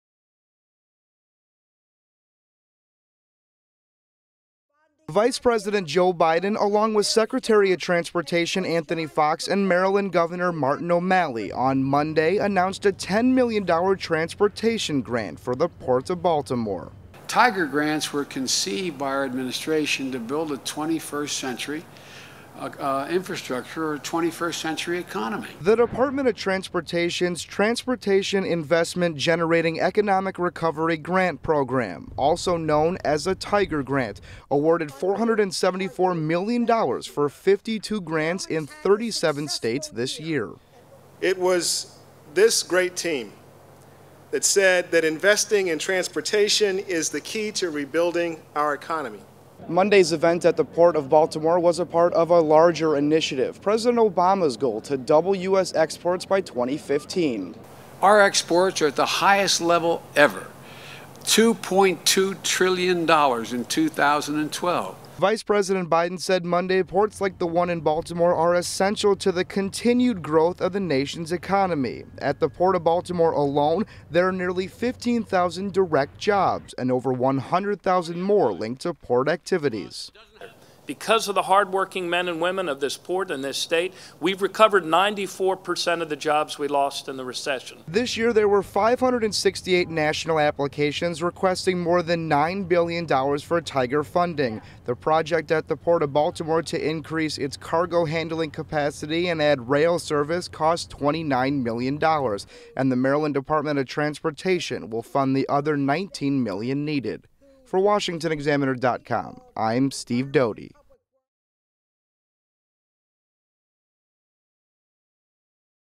VP Joe Biden announces $10m grant for Port of Baltimore
Vice President Joe Biden was joined by Maryland Gov. Martin O'Malley and others in announcing a $10 million TIGER grant to the Port of Baltimore. The grant was one of 52 grants awarded to 37 states, totalling $474 million.